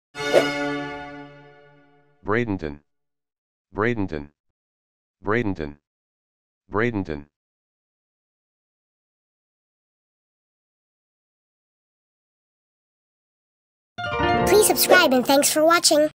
Bradenton (How to Pronounce Cities of the World)💬⭐🌍✅
This video is about "Bradenton". This video series is something special. We're covering the pronunciation of all the cities of the world.